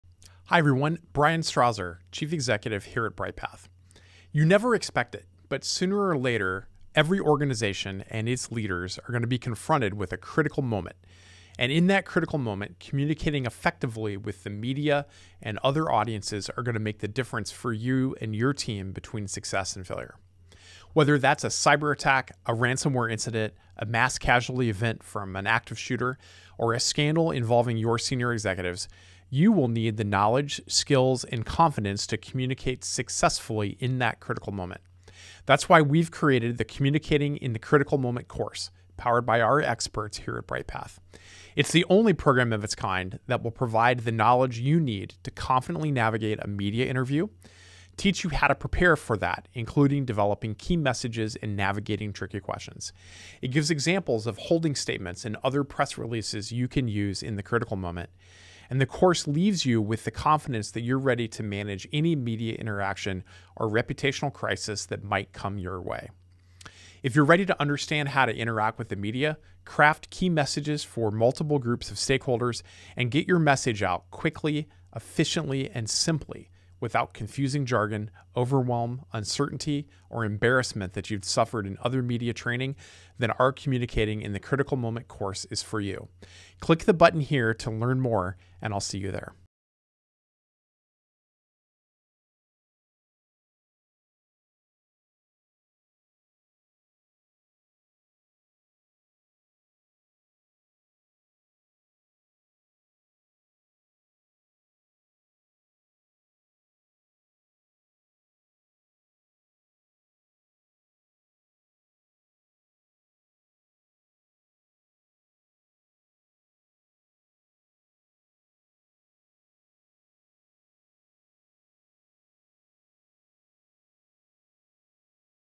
Bryghtpath: Communicating in the Critical Moment Course
No one ever expects it, but sooner or later, every organization and leader will be confronted with a critical moment in which communicating effectively with the media and other audiences will make the difference between success and failure.

Whether that critical moment is a cyber-attack, ransomware, a mass casualty event from an active shooter, or a scandal involving your senior executives, you will need the knowledge, skills, and confidence to communicate successfully.

Our Communicating in the Critical Moment course, powered by the experts at Bryghtpath, is the complete online course that prepares you to communicate confidently in the critical moment.